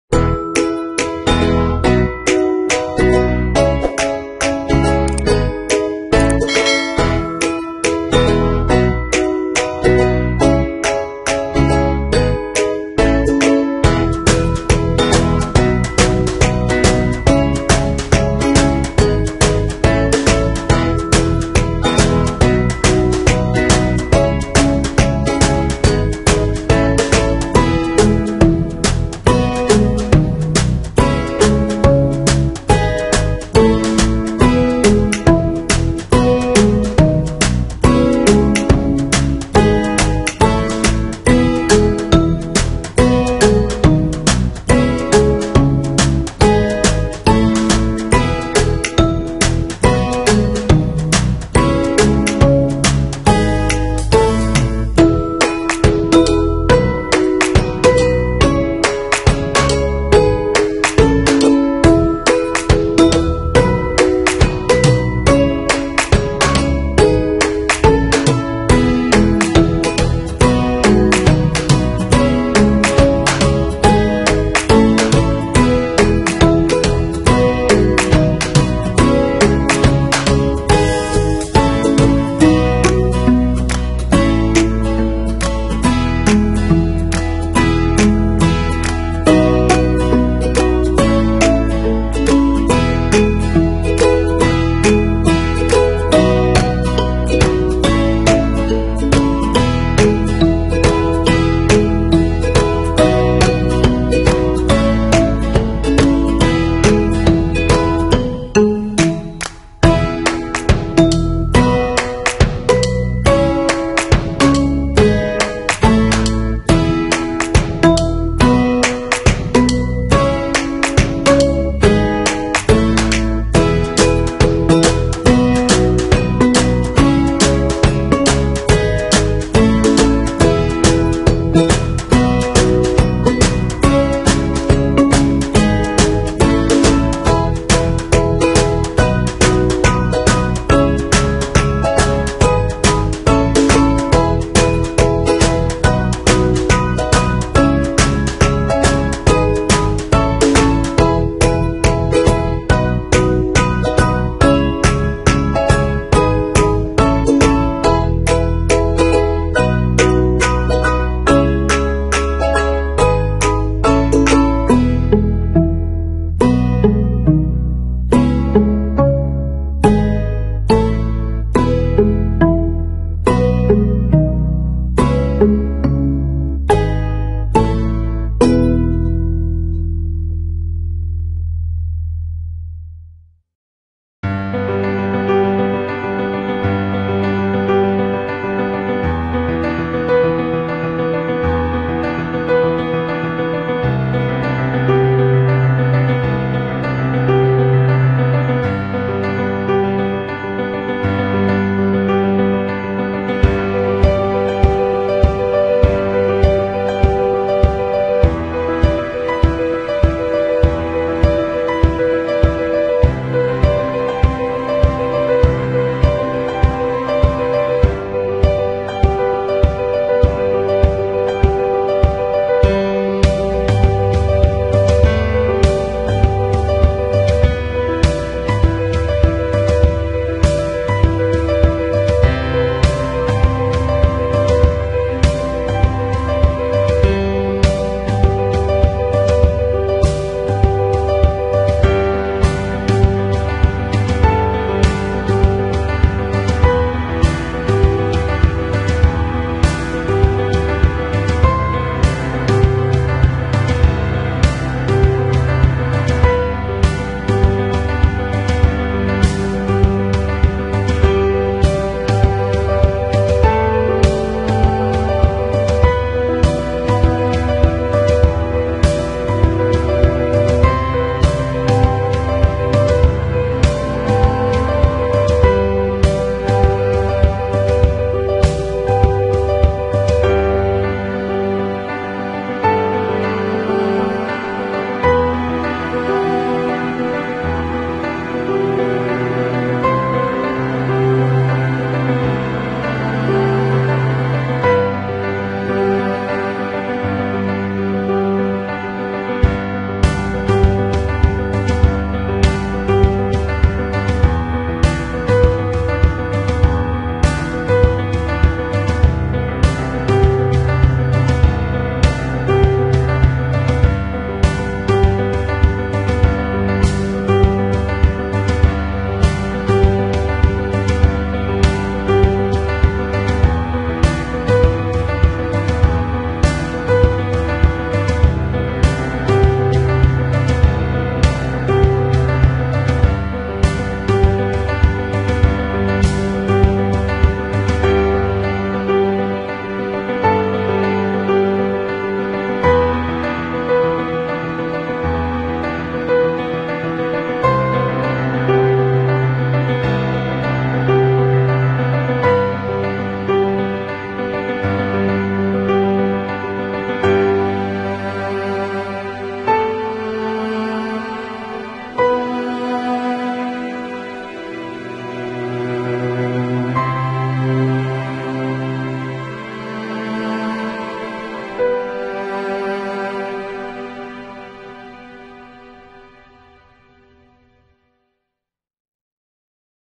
TEORI ASAM BASA MIND MAPPING
Video ini berisikan tentang mind mapping materi teori asam basa, dimana teori asam basa meliputi teori Arrhenius, teori Bronsted Lowry dan teori Lewis.